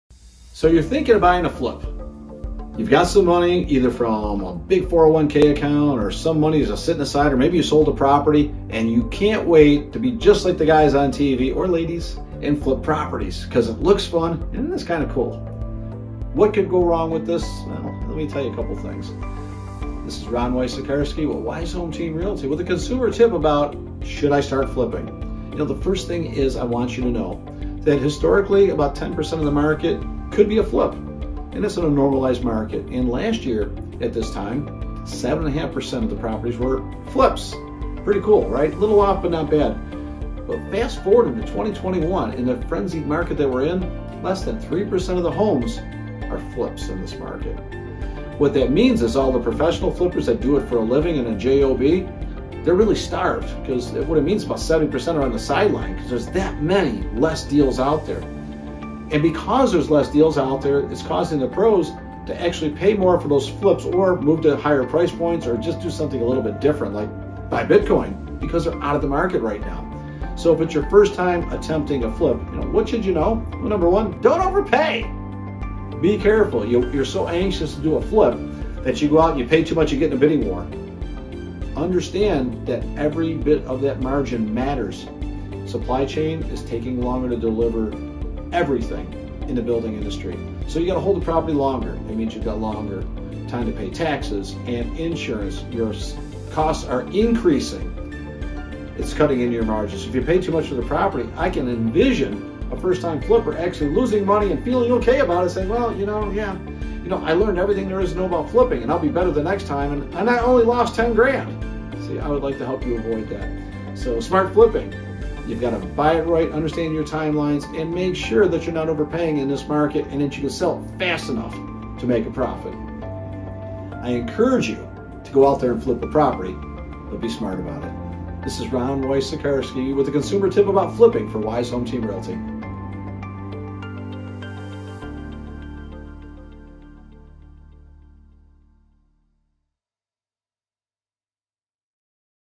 Should I Start Flipping Houses?!
We get this question a lot these days.

The market is on FIRE, and home prices are climbing up fast. So you're wondering.. should you start flipping? Could this be your ticket out of the daily corporate rat race?

Today, we're answering your burning questions about flipping.

Give Ron a shout!